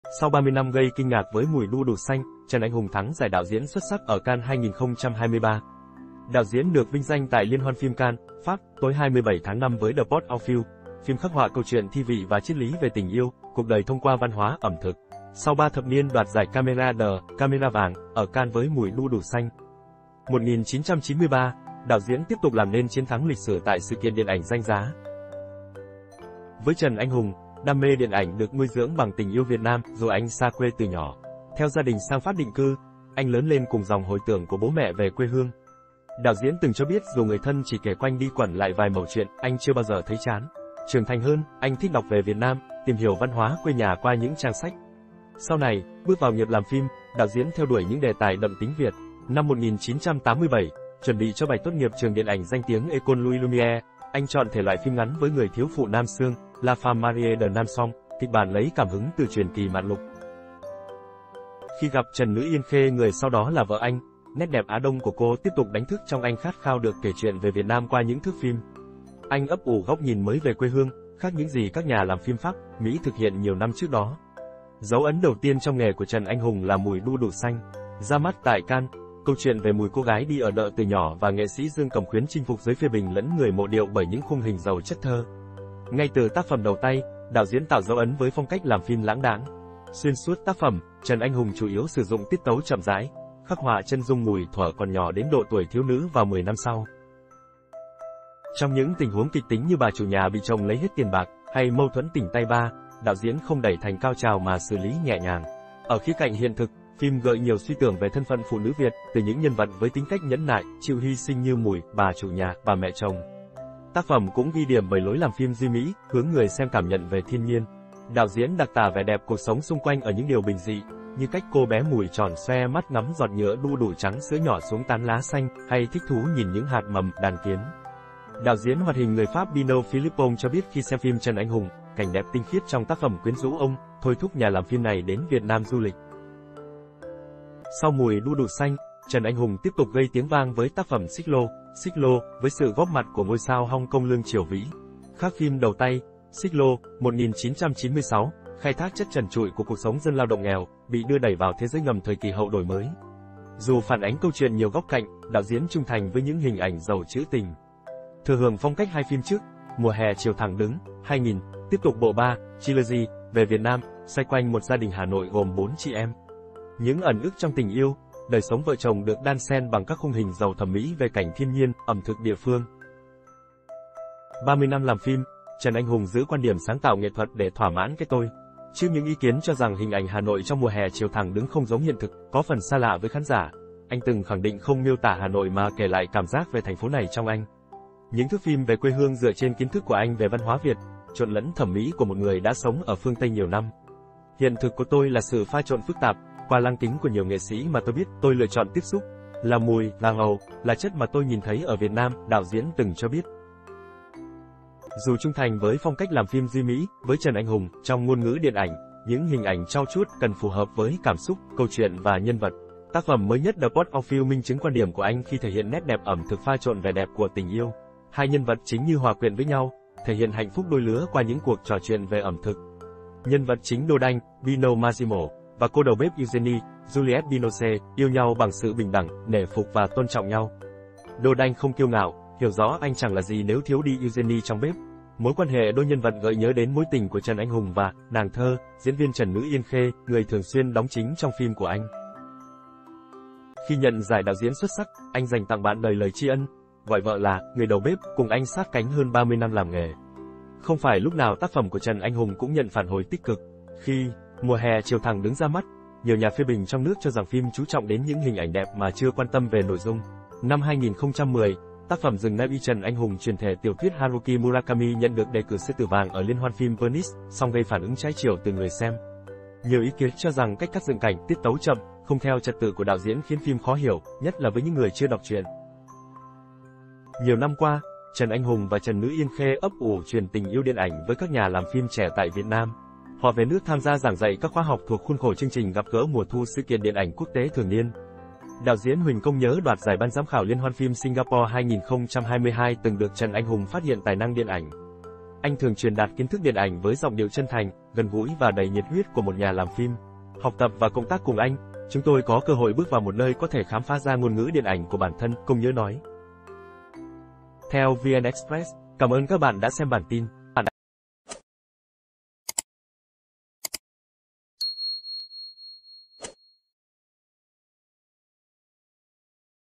ĐẠO DIỄN TRẦN ANH HÙNG - TỪ 'MÙI ĐU ĐỦ XANH' ĐẾN ĐẠO DIỄN XUẤT SẮC CANNES
Đạo diễn được vinh danh tại Liên hoan phim Cannes (Pháp), tối 27 tháng 5 với The Pot au Feu, phim khắc họa câu chuyện thi vị và triết lý về tình yêu, cuộc đời thông qua văn hóa ẩm thực. Sau ba thập niên đoạt giải Caméra d'Or (Camera Vàng) ở Cannes với Mùi đu đủ xanh